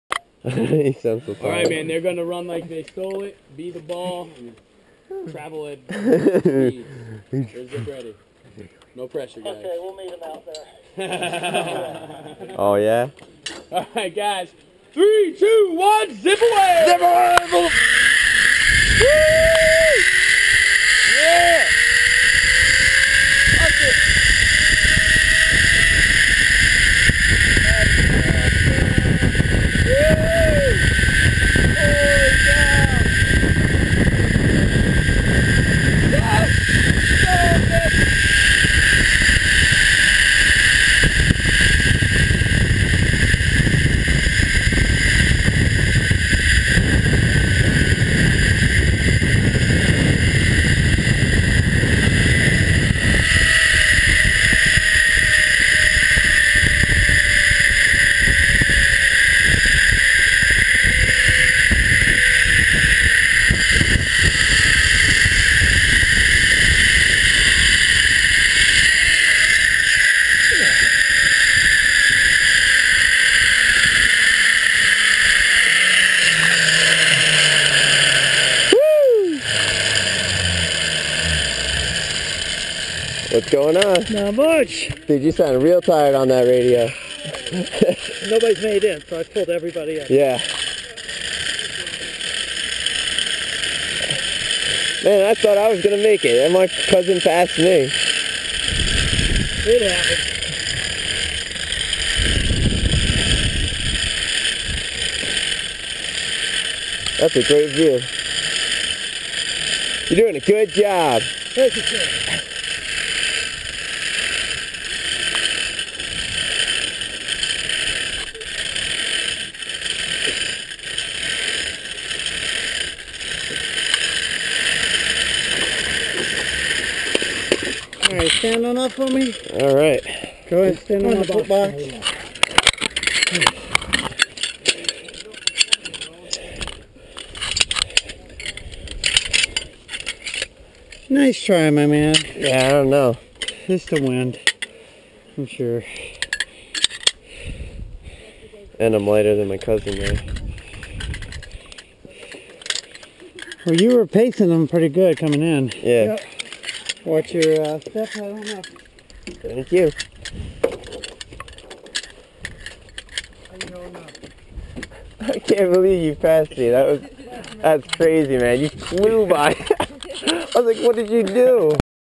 good zip vipeo 4
racing my cuz thought i had him beat.